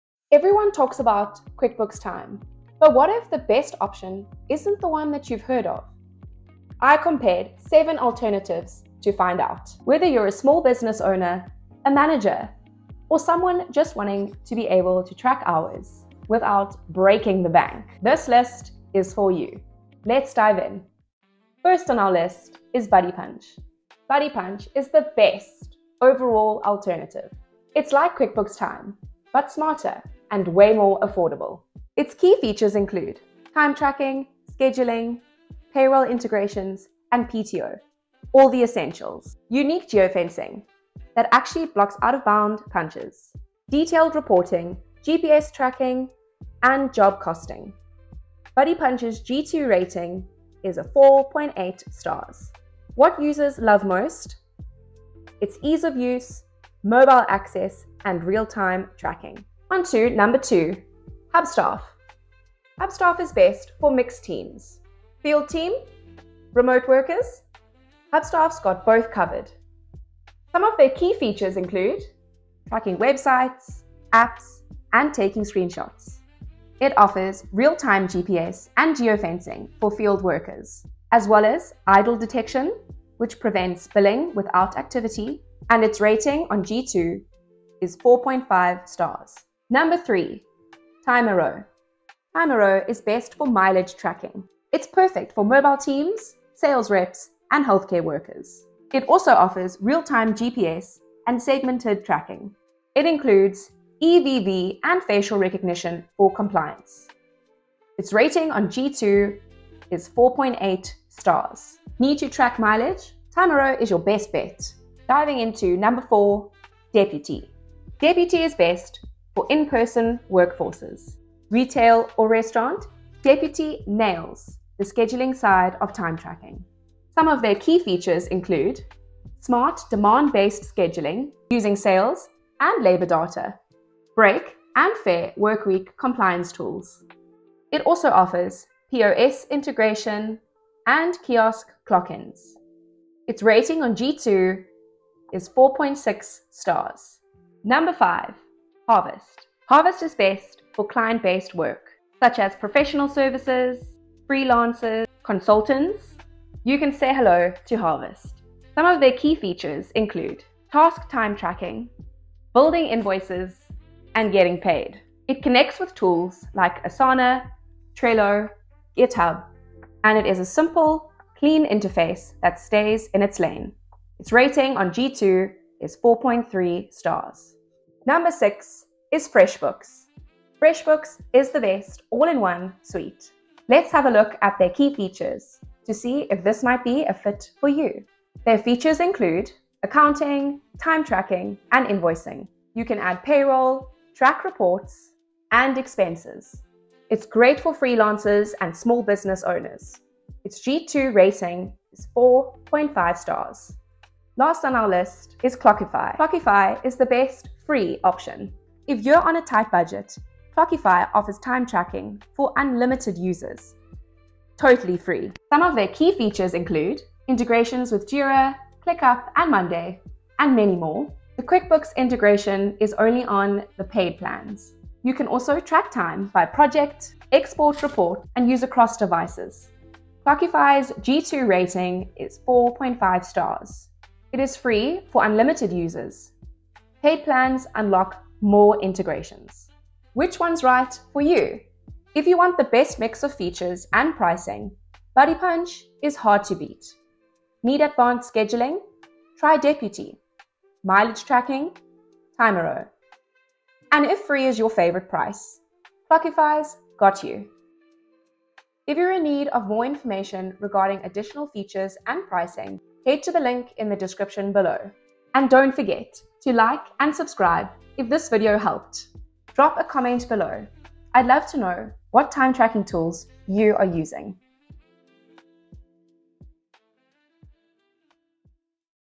The 7 Best QuickBooks Time Alternatives (Lower Cost & More Features)
QuickBooks Time is a popular time tracking tool, but its high cost and requirement for a QuickBooks Online subscription make it impractical for many businesses. We review the 7 best QuickBooks Time alternatives, focusing on tools that offer comparable features, lower pricing, or unique specialization for different workforce needs.

Tools Included in This Review:

1) Buddy Punch: This is the best overall alternative, offering almost all the same features as QuickBooks Time at nearly half the cost, with advanced geofencing that actually prevents off-site punching and instant integration with QuickBooks Online and Desktop and other payroll programs.

2) Hubstaff: This is the best alternative for blended workforces, offering dual functionality by monitoring field employees with GPS tracking and geofencing, while tracking computer-based workers using app/URL tracking, automatic screenshots, and idle detection.

3) Timeero: Best for mileage tracking, this employee GPS tracking app features a unique segmented tracking tool that automatically allocates time toward specific jobs by combining real-time GPS tracking with geofences, eliminating the need for employees to manually clock in and out.

4) Deputy: This is the best alternative for in-person businesses, offering robust scheduling features like demand scheduling (using historical data), smart scheduling (connecting sales data to labor costs), and break/fair workweek compliance tools.

5) Harvest: Best for professional services, this tool focuses on tracking time toward client projects and creating accurate invoices, allowing you to easily integrate with project management platforms like Asana and quickly turn tracked data into client invoices.

6) Freshbooks: The best alternative for replacing your entire QuickBooks suite, this product includes both accounting and time tracking features, offering everything from tracking expenses and mileage to generating invoices and accessing detailed financial reports.

7) Clockify: This is the best free alternative, providing basic time tracking features for unlimited users and unlimited projects at no cost, and it supports integration with multiple project management systems like Jira and ClickUp.

Get an overview of our top picks in this video, and check out our blog post for more details on each system's features and pricing